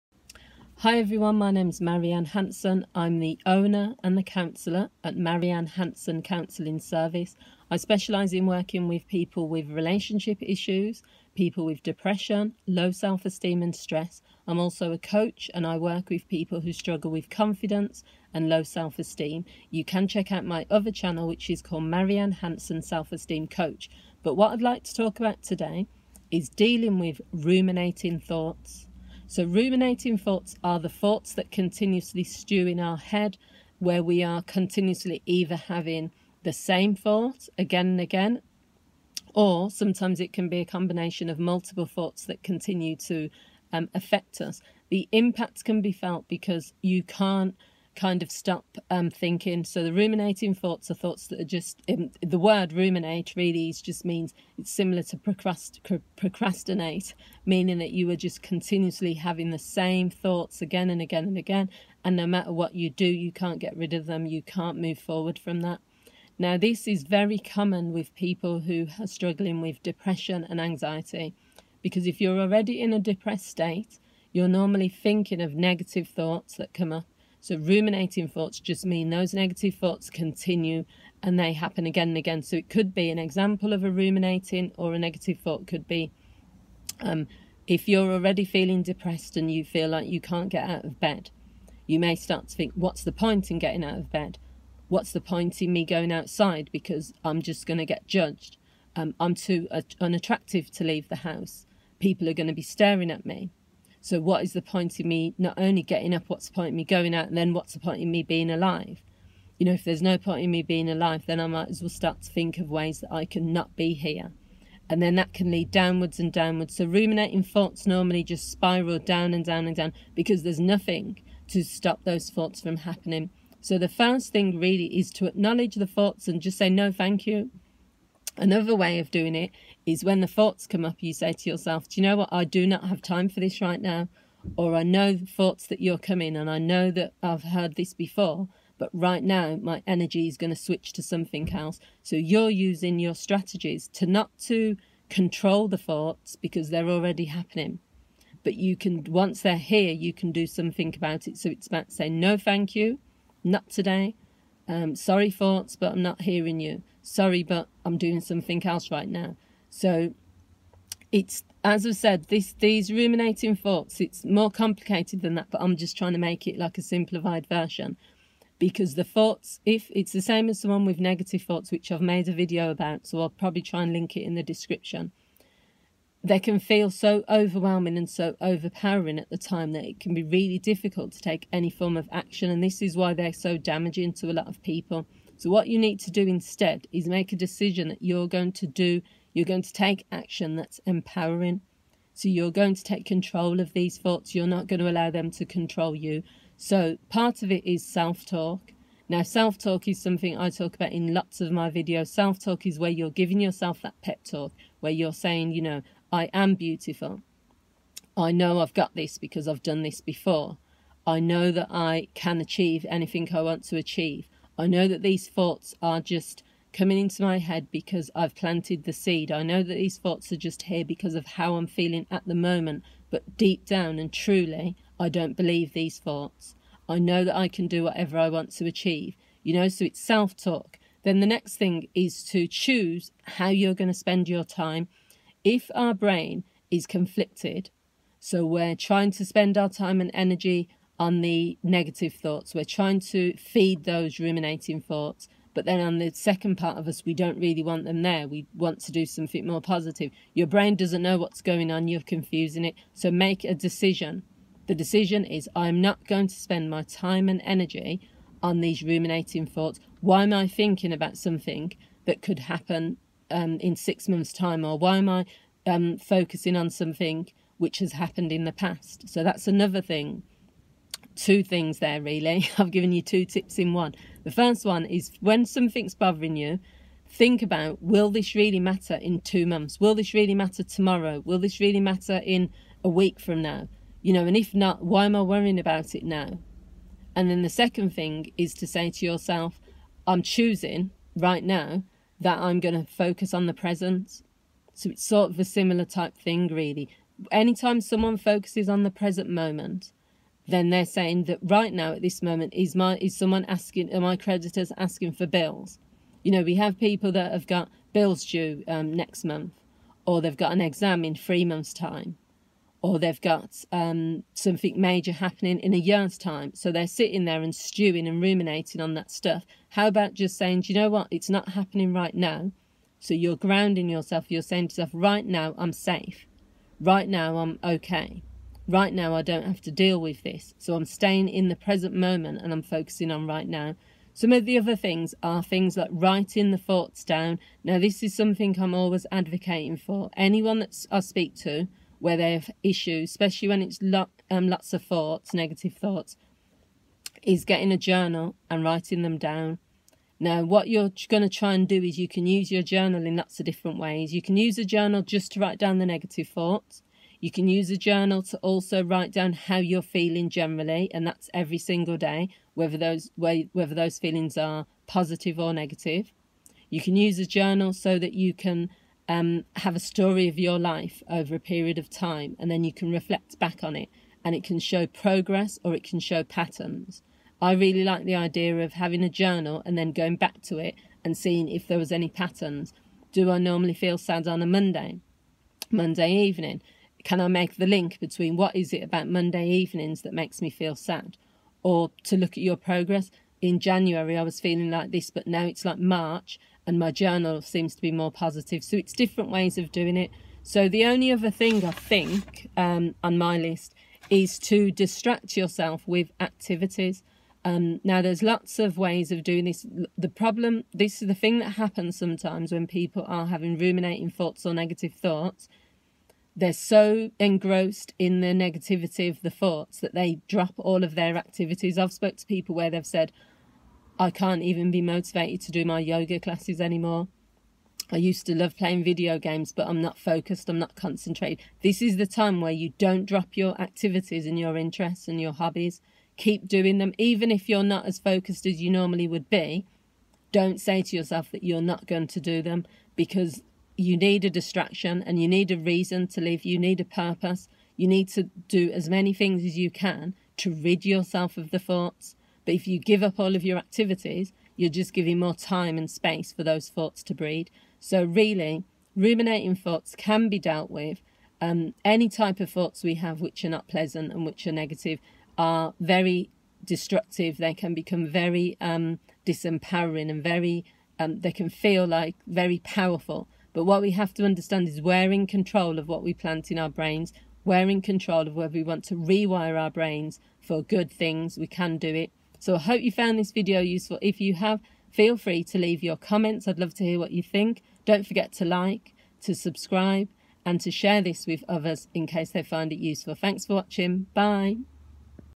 Dealing With Ruminating Thoughts
Ruminating thoughts are continuous, rambling, repetitive thoughts that can occupy a person’s mind. In most cases, an individual is unable to stop the thoughts or to reframe them into positive ones. This can lead to a feeling of emotional exhaustion, overwhelm, and distress. In this video, I provide more information about ruminating thoughts and the impact that they can have on a person’s life.